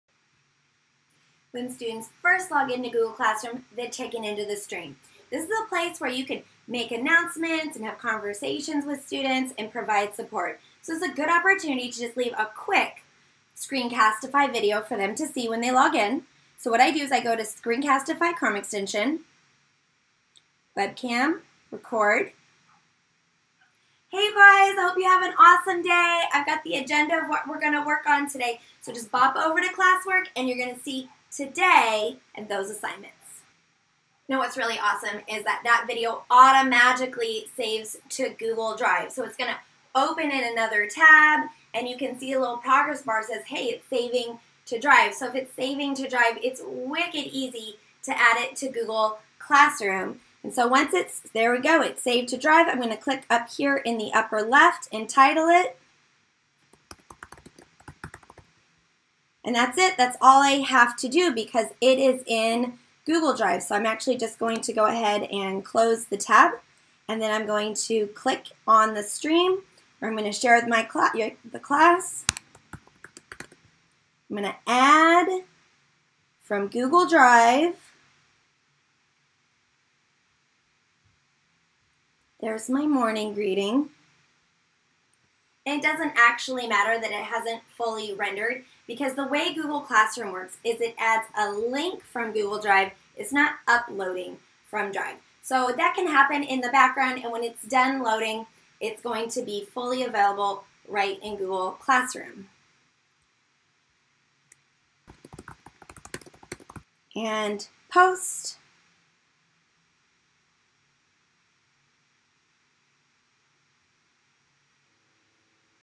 Adding a quick Screencastify video to the Stream of Google Classroom
Screencastify saves to Google Drive. It's important to have yourself in your digital materials so just jump in and say a quick hello using the Screencastify Chrome extension. Add the video right from Google Drive. Super easy.